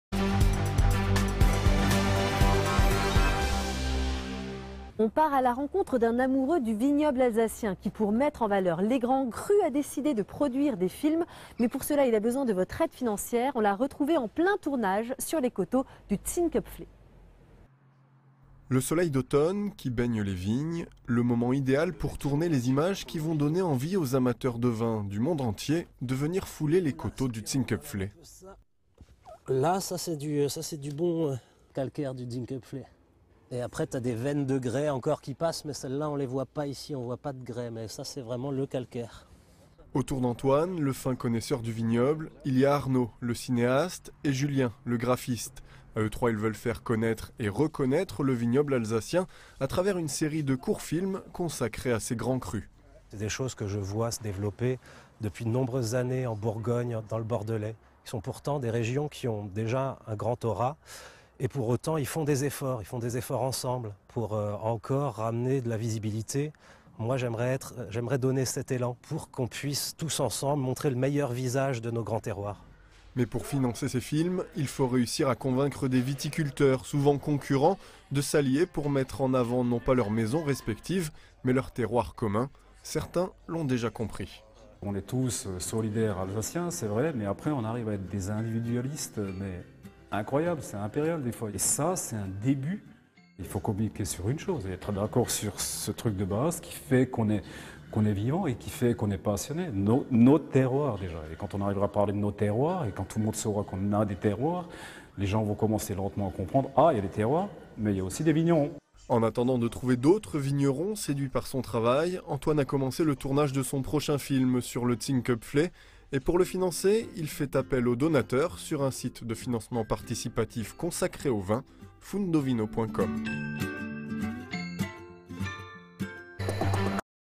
France3 Alsace et Antoine MANTZER porteur d'un projet sur Fundovino.
Reportage de France 3 Alsace sur Antoine MANTZER porteur d'un projet sur Fundovino.